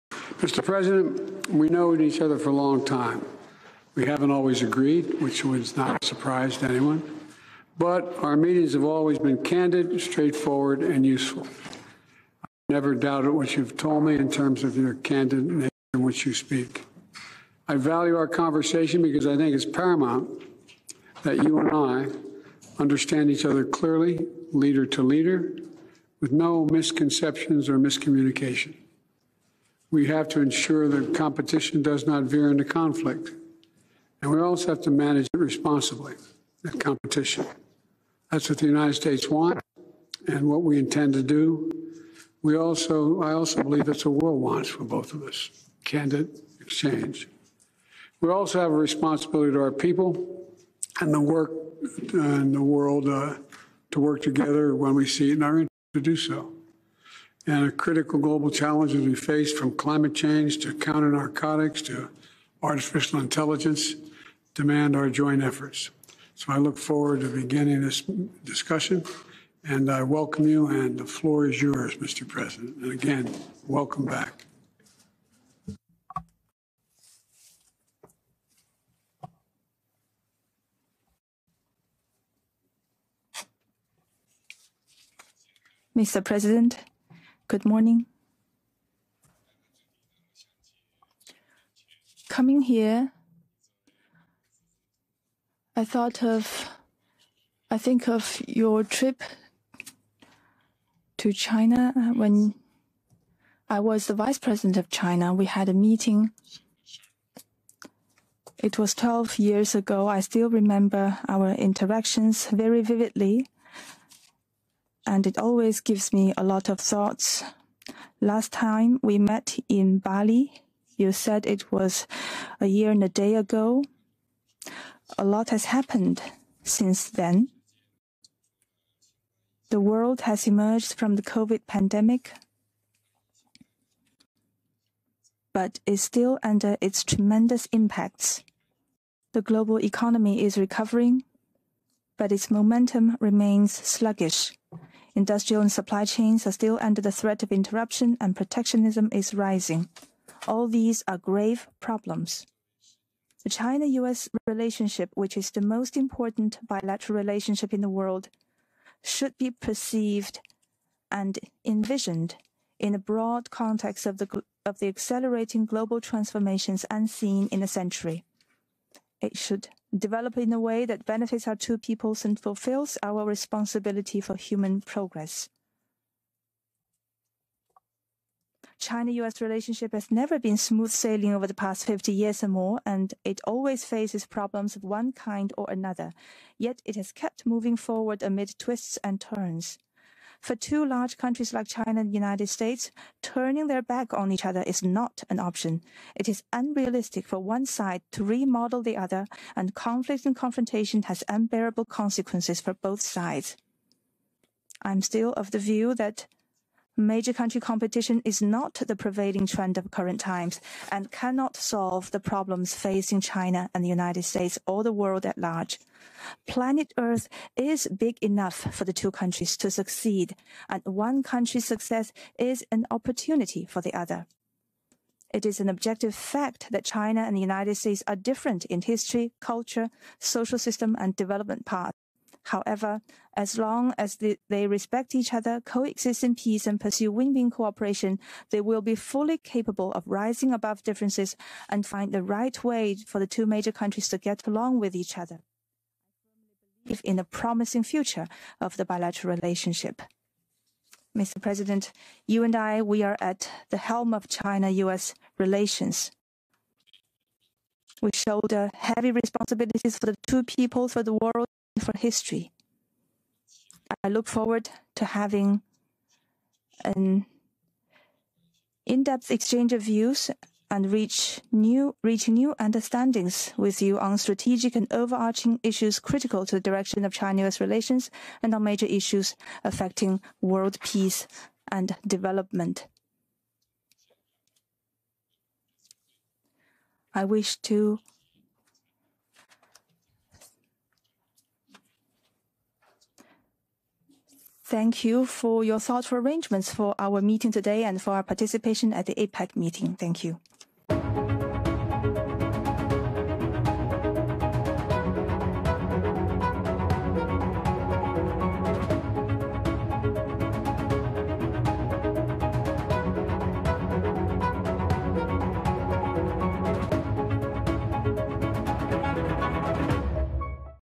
See President Biden go face-to-face with China’s Xi in high-stakes meeting
President Biden met with Chinese President Xi Jinping in Woodside, California amid rising tensions between the United States and China. In this video, you go inside the meeting between the two leaders with Biden saying, "I value our conversation because I think it’s paramount that you and I understand each other clearly, leader to leader, with no misconceptions or miscommunication.” Xi saying through an interpreter, "For two large countries like China and the United States, turning their back on each other is not an option.”